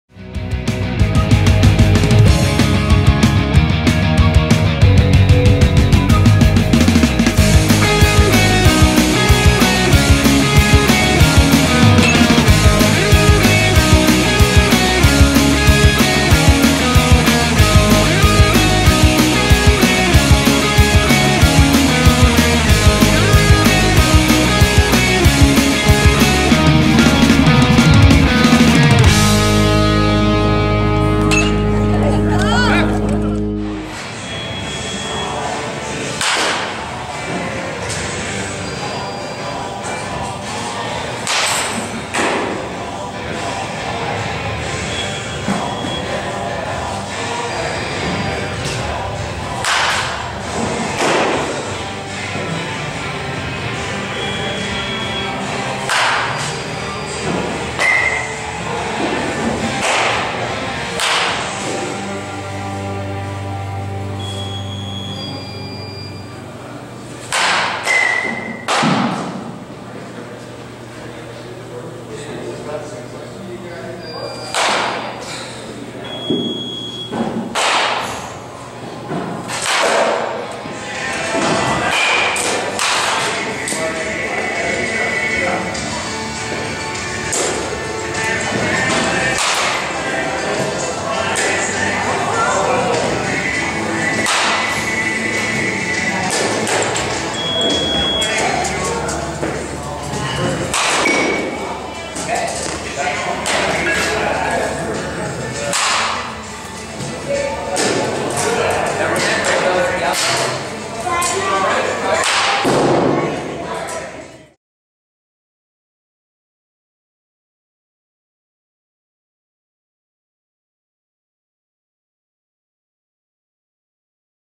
Connor Underwood Hitting Video March 13
El Segundo's Conner Underwood. 6'1 190lbs
Class of 2014
1B/C/3B
Throws: Right - Bats: Right
Hitting Video from March 20013